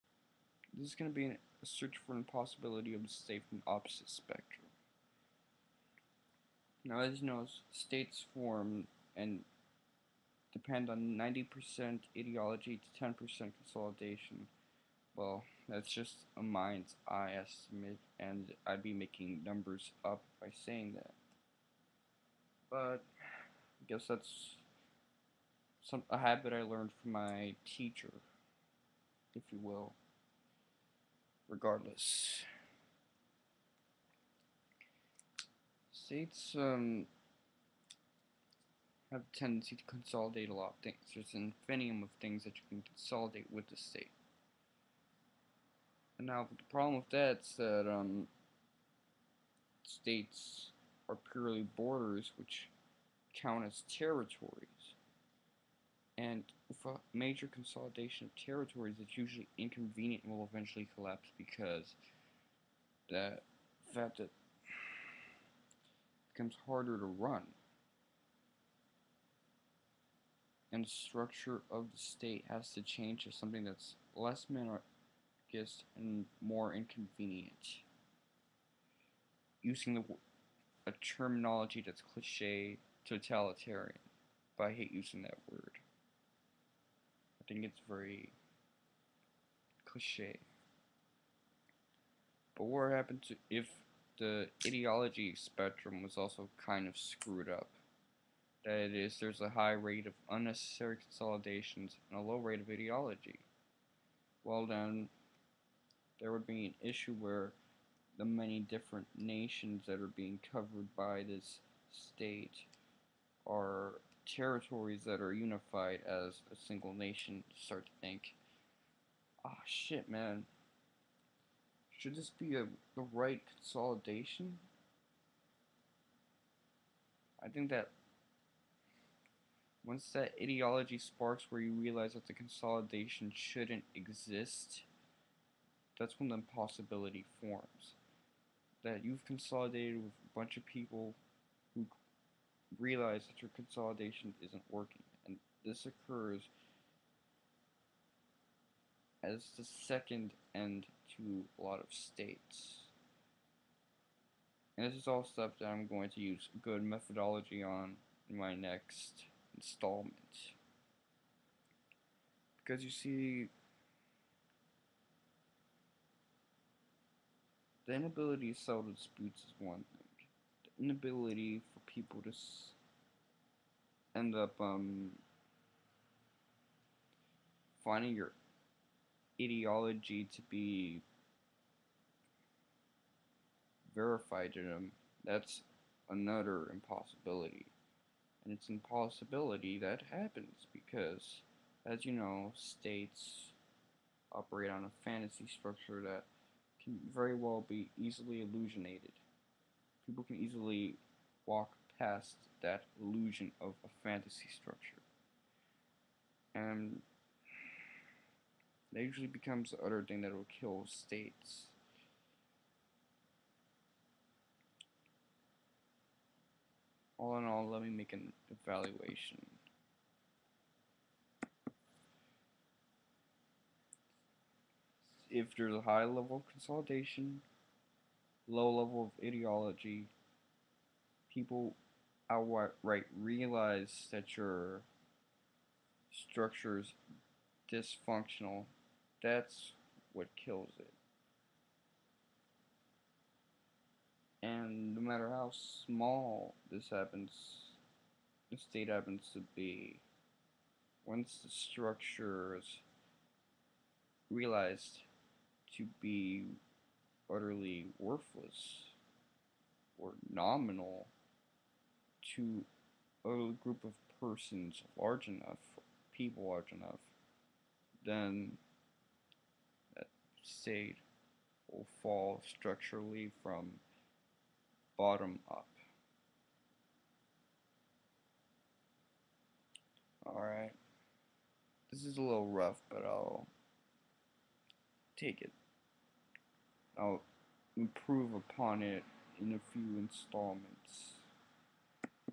Low Ideology + High Consolidation = Another Impossibility
I have to make a few connectiions to minarchism, this is a rough idea.